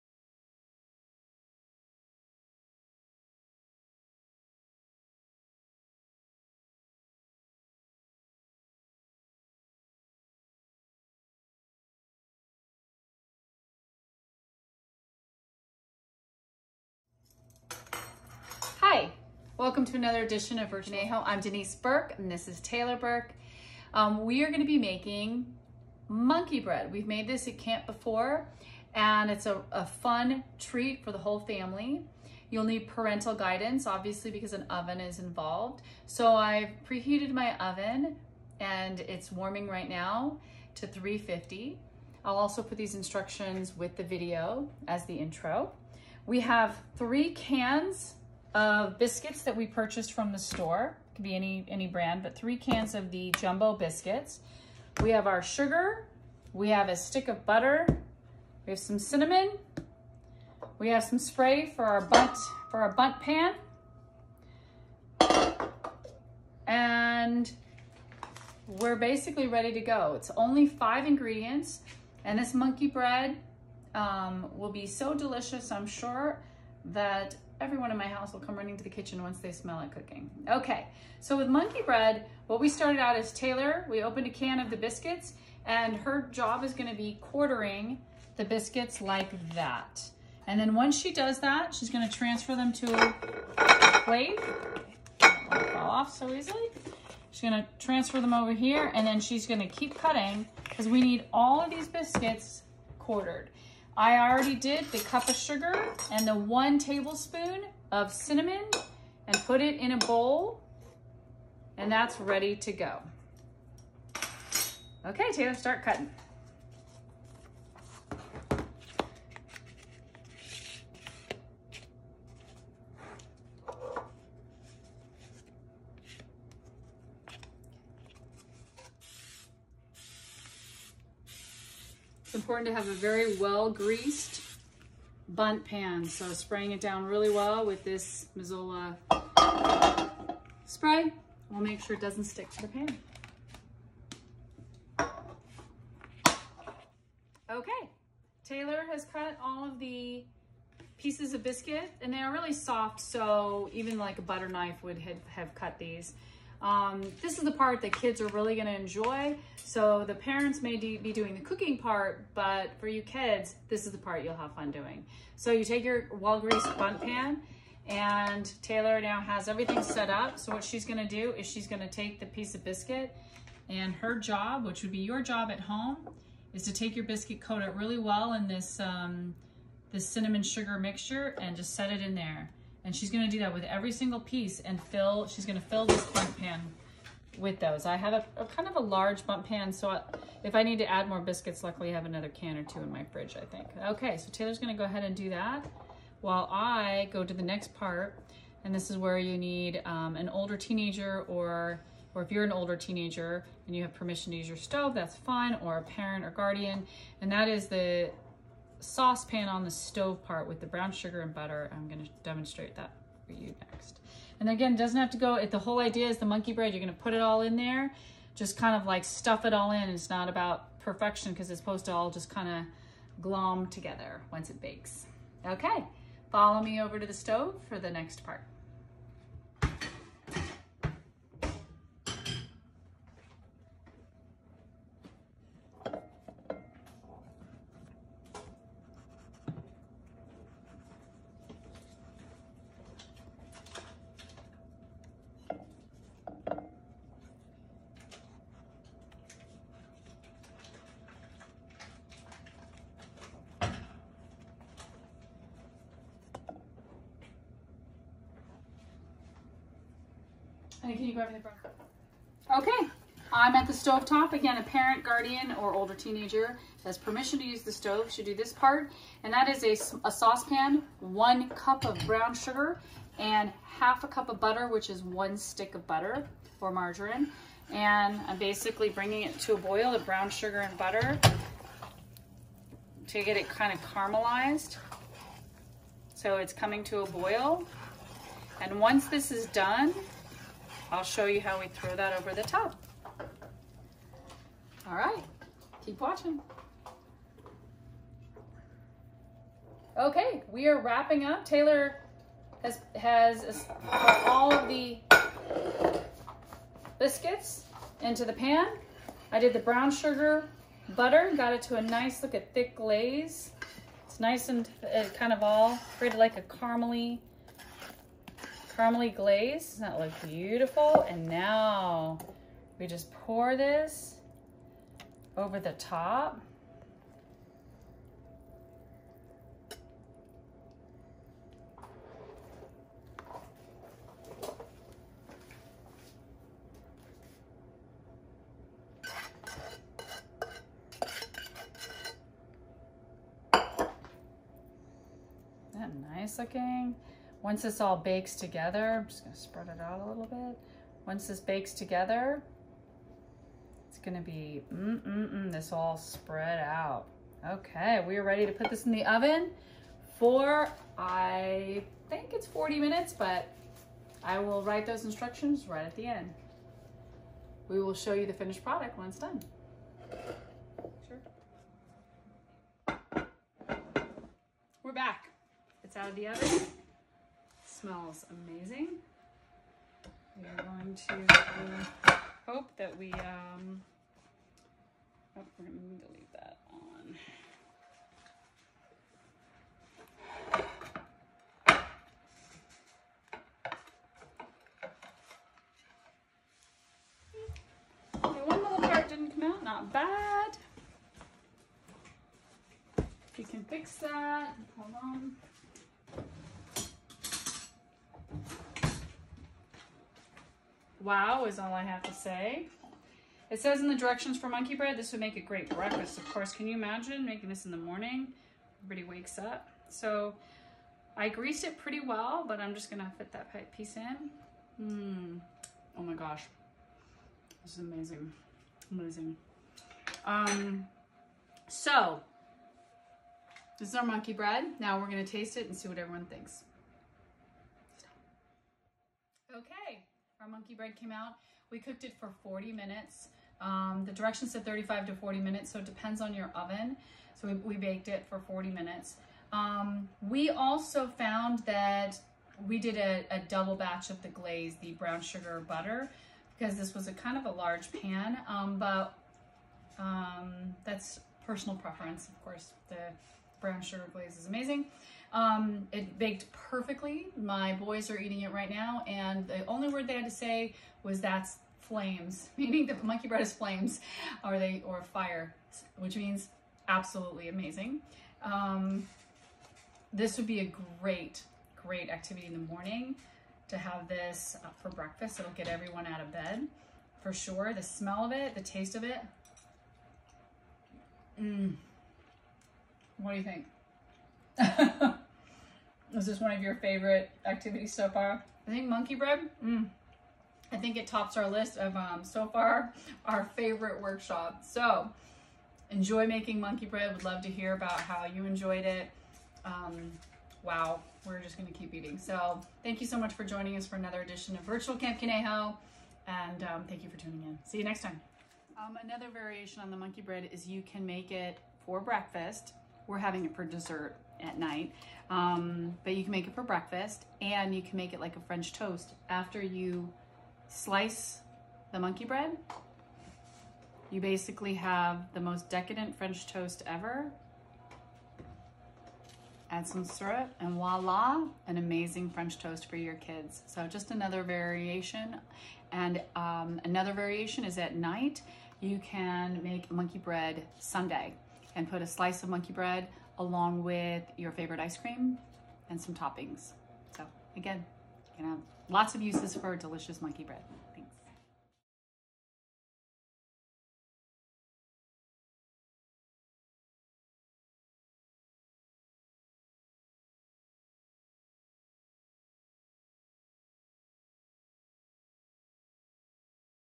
Delicious Monkey Bread
Denise and Taylor demonstrate how to make delicious Monkey Bread at home. This workshop requires adult supervision since baking is involved. Ingredients: 3 large cans refrigerated biscuits, cinnamon, sugar, butter, brown sugar.